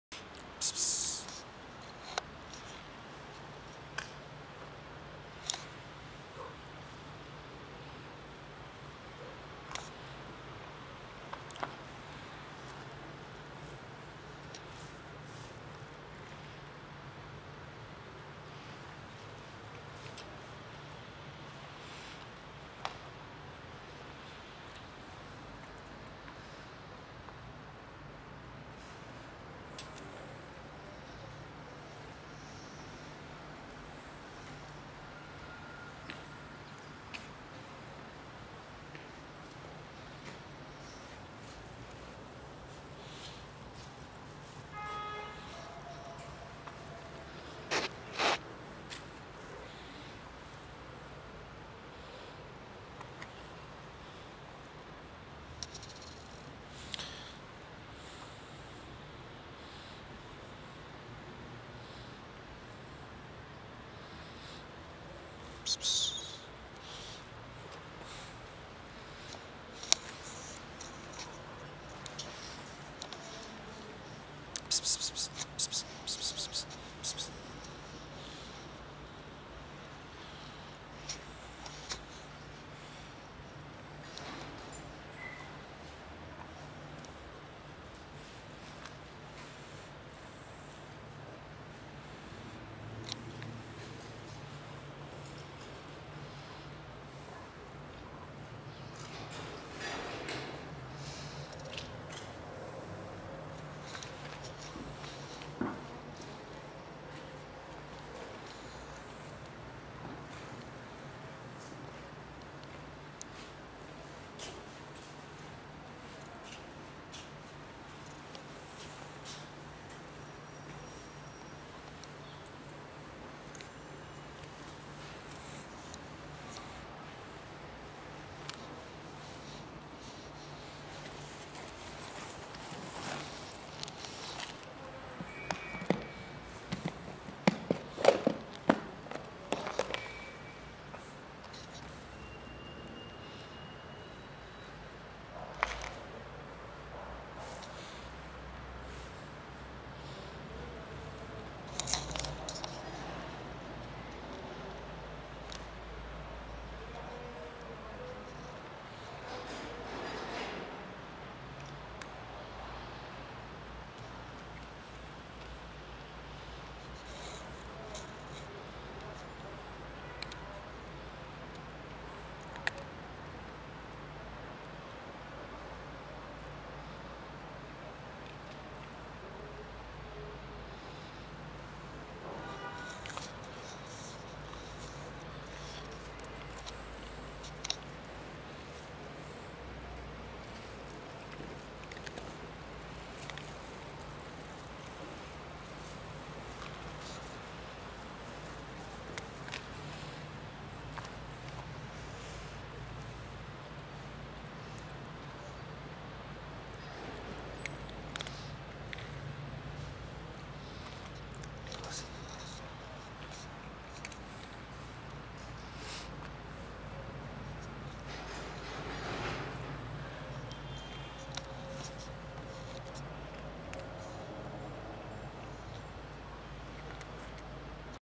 This cat was acting cute to seduce me to feed her
A very fine cat was sitting alone and asked for food. I gave food.